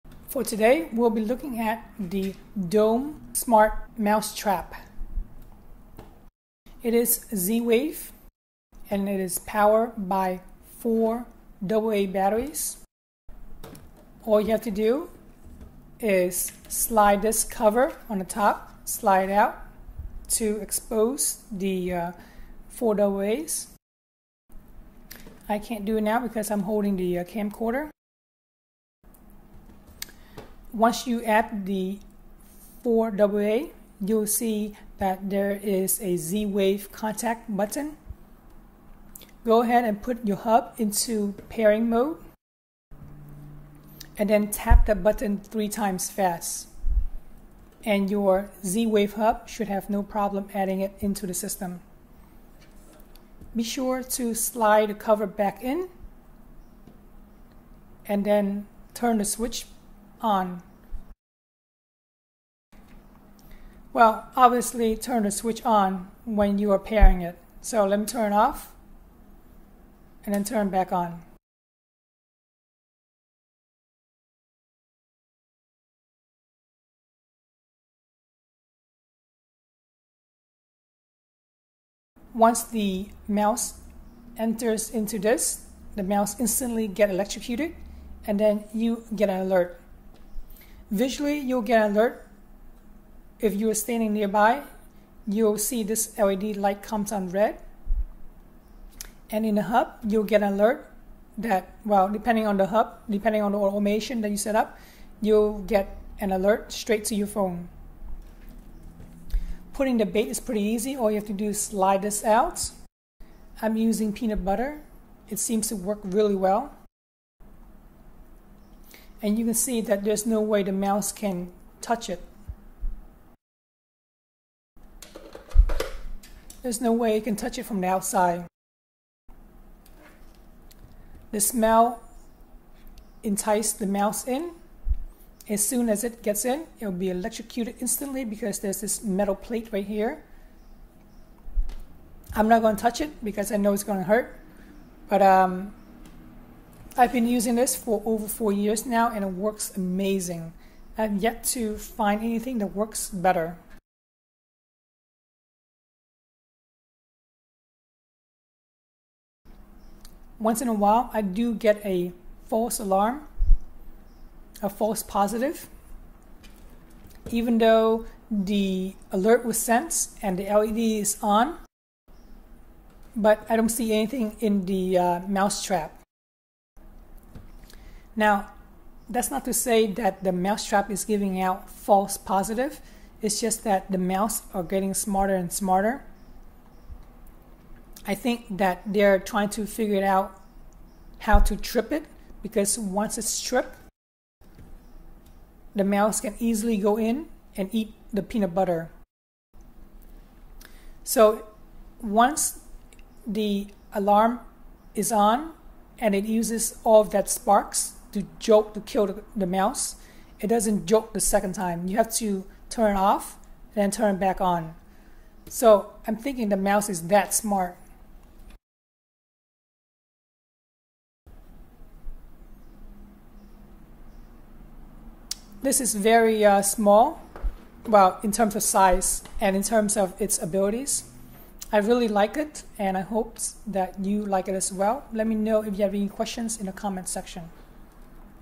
dome mouse trap zwave smart electric trap review
Finally, a mouse trap for the 21st century! Kills mouse by zapping it and alerts you instantly! No more forgetting to check the mouse trap and having to smell that horrible smell of dead mouses in the house.
Pros
+ small and compact
+ instant alert when trap has been tripped by a mouse
+ uses 4 AA batteries

Cons
- no audible alert, small LED to alert
- cannot zap multiple times, must re-arm after each zap.

+ Dome zwave mouse trap
+ AA lithium rechargeable batteries

+ DJI OM4 stabilizer
+ Samsung S21
+ Zoom portable mic
+ Davinci Resolve Studio